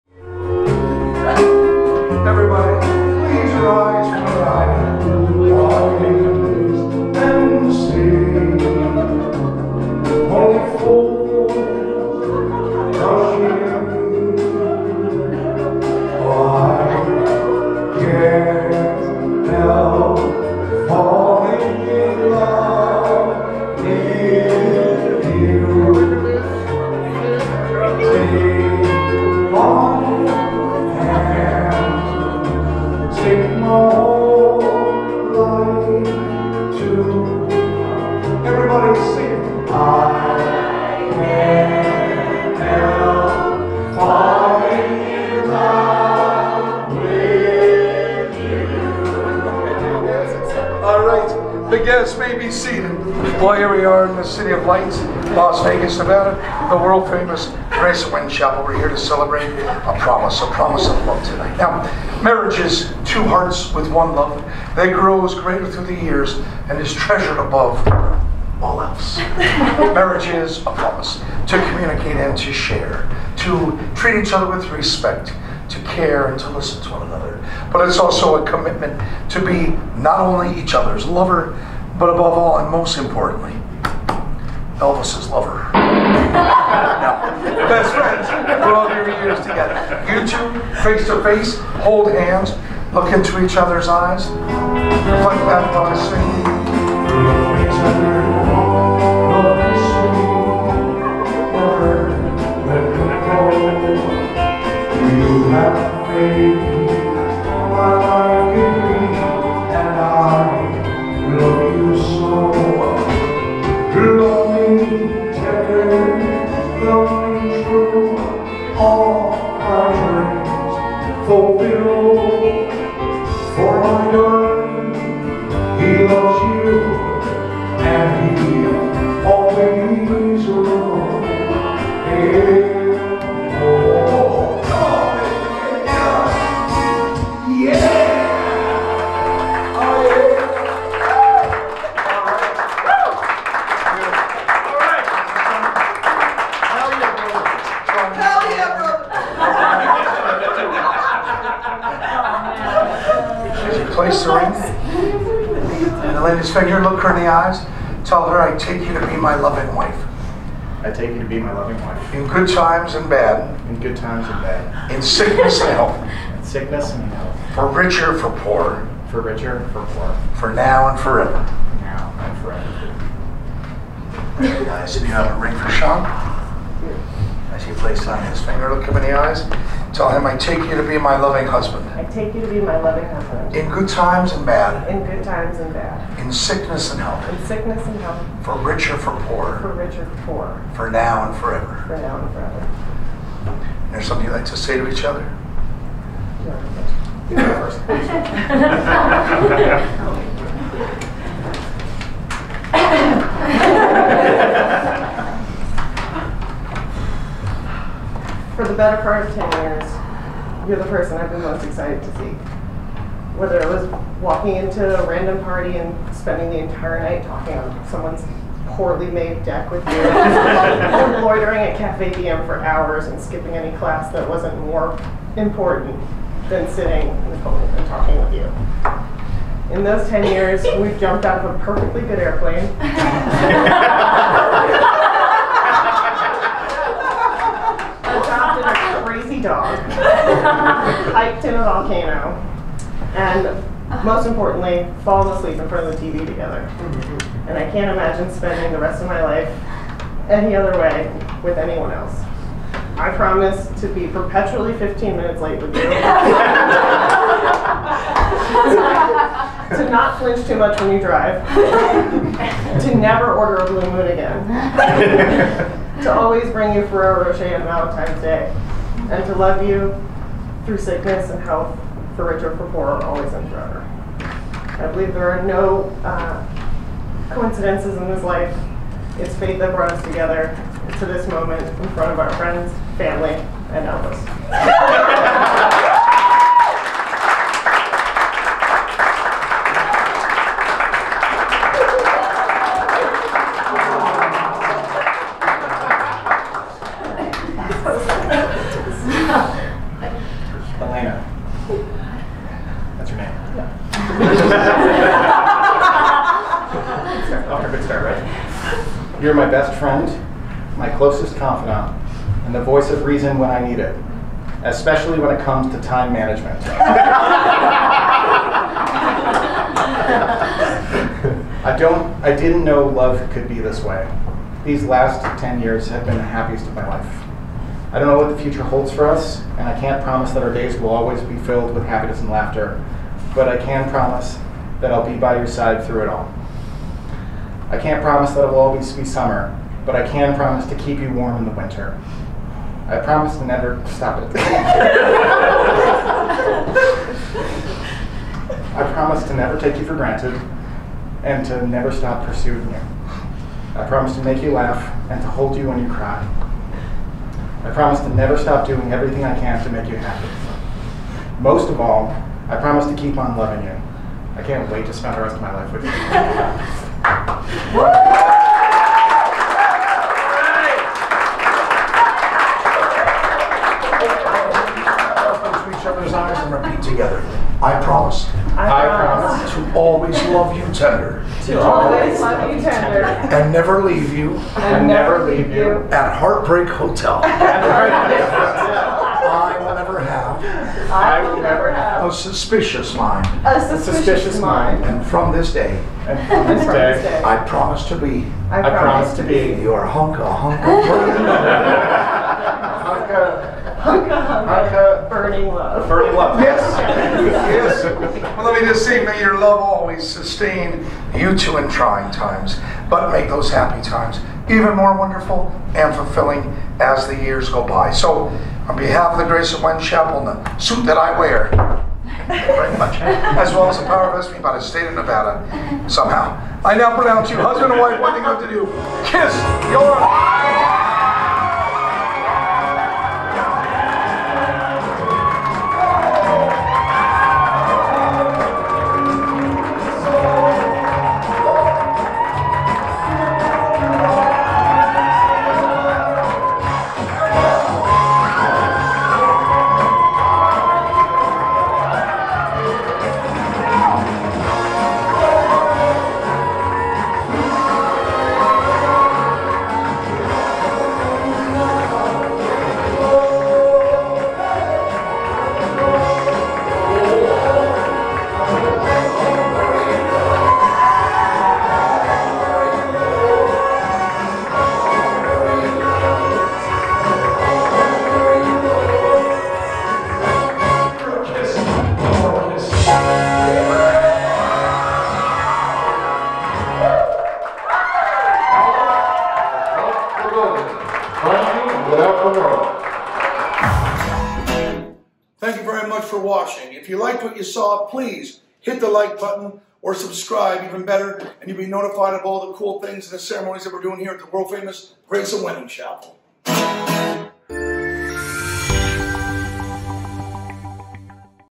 Elaina & Sean's Elvis Wedding in Las Vegas | Graceland Chapel
Elaina & Sean's Elvis Wedding Ceremony at Graceland Chapel in Las Vegas Nevada.

Congratulations!

Home of the World's First Elvis Themed Wedding!